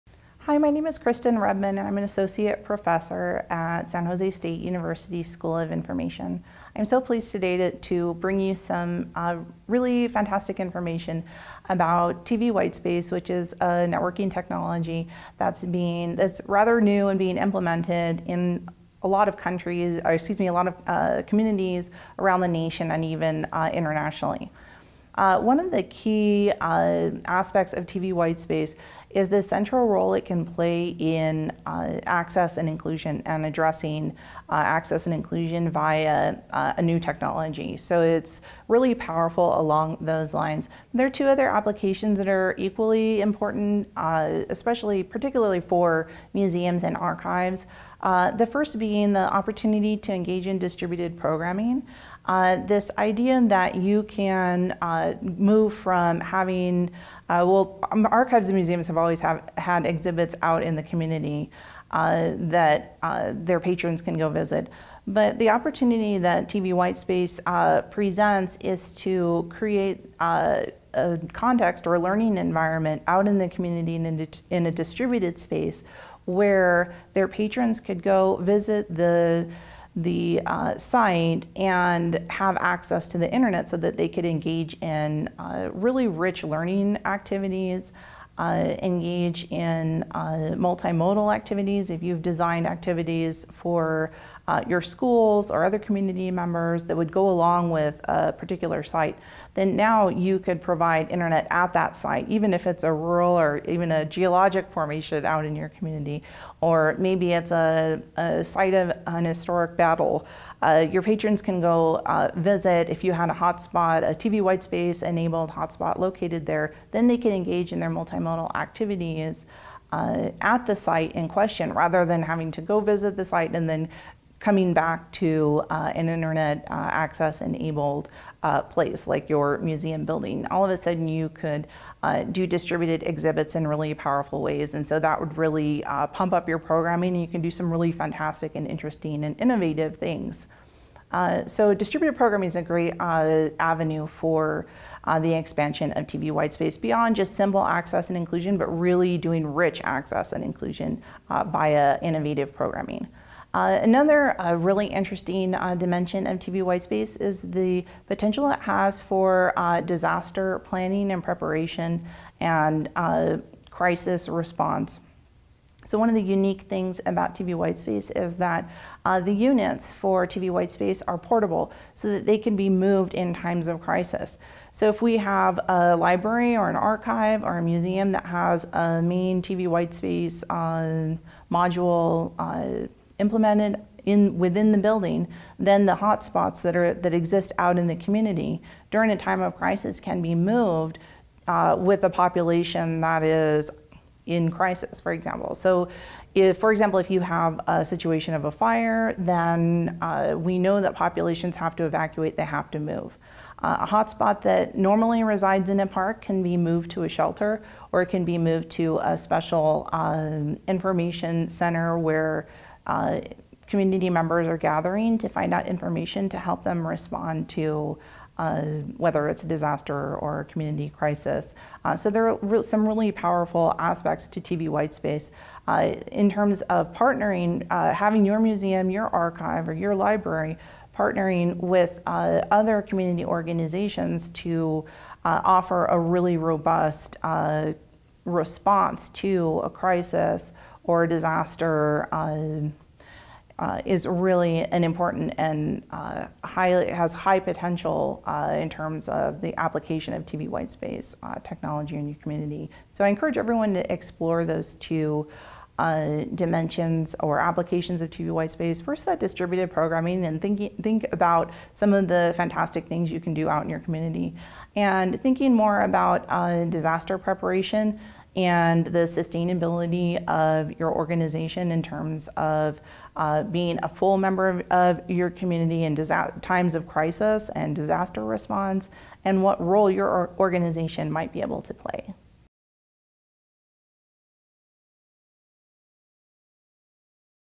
Applications for TV WhiteSpace
In this video, Kristen Rebmann, Associate Professor at San Jose State University's School of Information, discusses two key aspects and applications of TV WhiteSpace technology: 1) the benefits and opportunities for museums and archives such as distributed and innovative programming, and, 2) the potential for disaster planning and preparation crisis response by partnering with other community organizations. This video supplements the session, "Innovative Applications of TV Whitespaces in Archives, Libraries, and Museums" given at the 2017 ATALM annual conference held in Santa Ana Pueblo, New Mexico, on October 10-13, 2017.